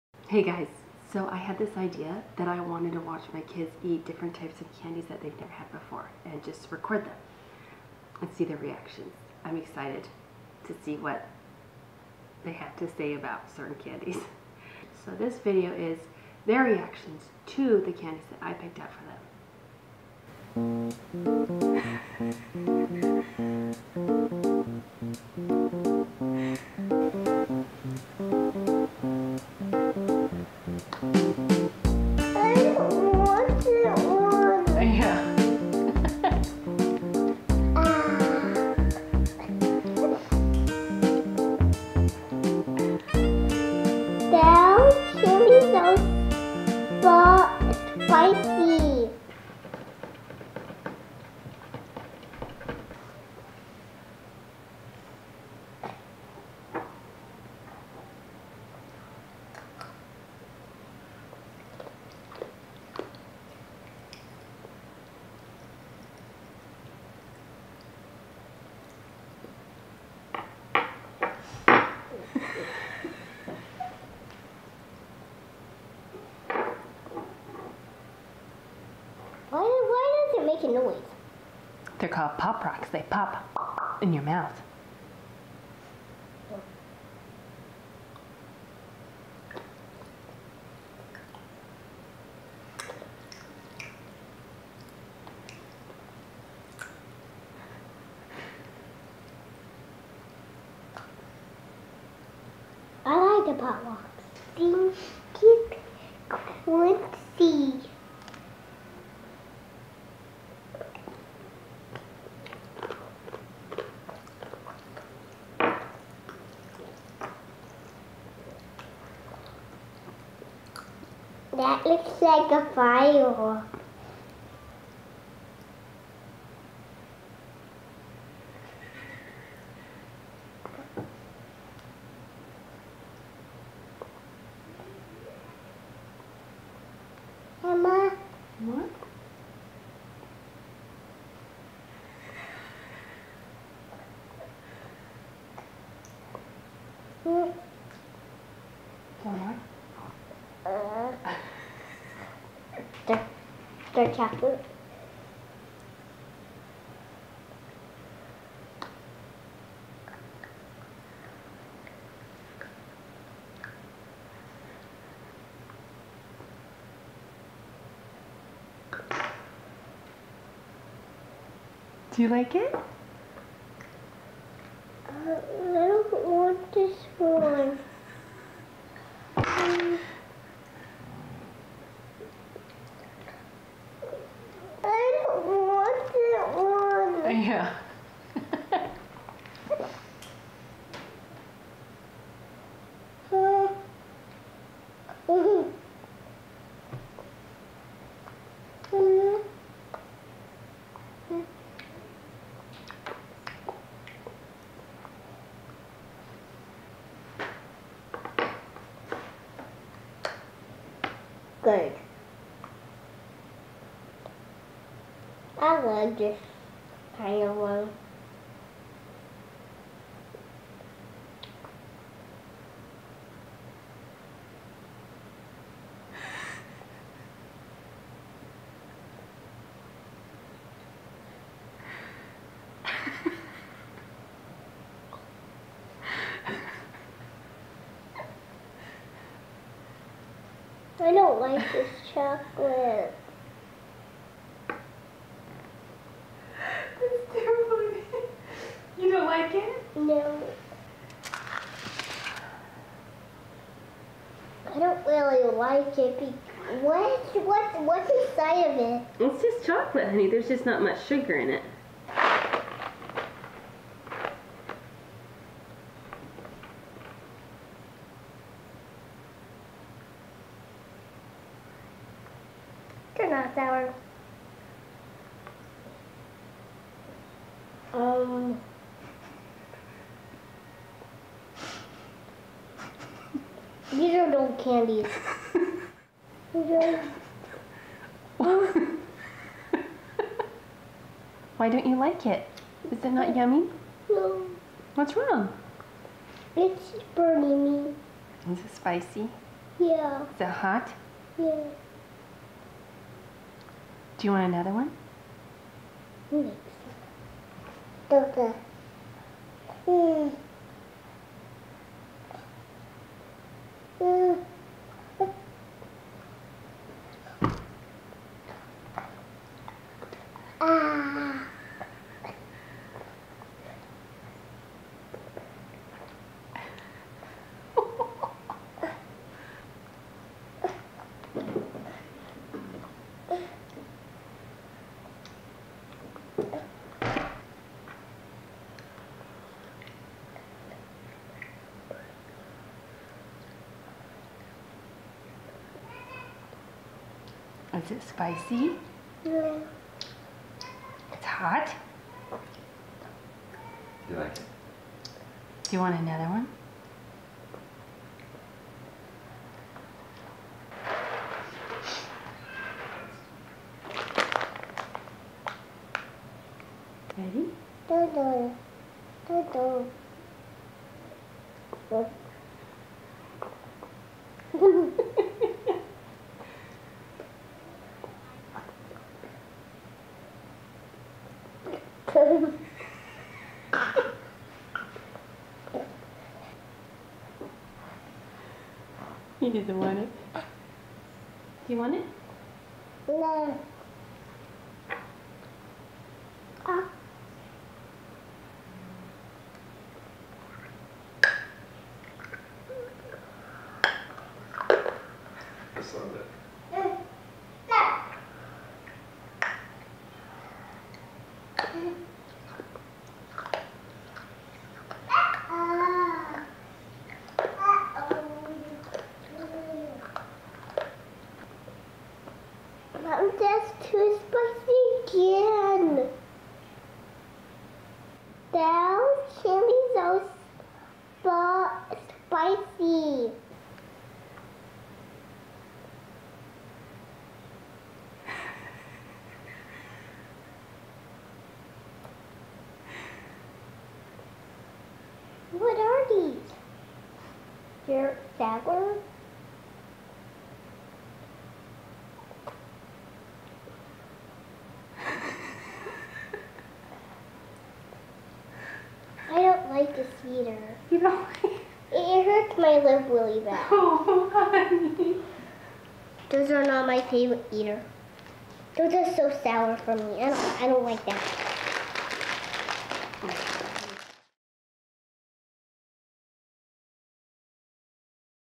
Kids funny reaction to eating candy
With all of the Halloween candy I had an idea of having the kids try candies they've never tried before. I have to admit, it was pretty entertaining. I had so much fun filming this video (I apologize for all the laughing, I couldn't help it...). ENJOY!

Family Introduction:
Mom: Rachel
Dad: Jim

Harmony: 4
Jimmy:3
Reed: 1